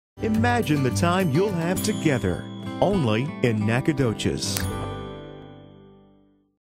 Discover the Mast Arboretum...Only in Nacogdoches!
The oldest town in Texas is a great place to spend a romantic weekend. Redefine: "Honey Do List" as you enjoy an easy stroll through the Mast Arboretum on the accessible campus of Stephen F. Austin State University. Hike and bike miles of scenic trails, discover experimental and native gardens, shop in one-of-a-kind stores, enjoy live local music and stay in a landmark hotel or historic bed & breakfast retreat. Imagine the time you'll have together. Only in Nacogdoches.

Request a free visitors' guide today!

Featured attractions: Mast Arboretum and Ruby M. Mize Azalea Garden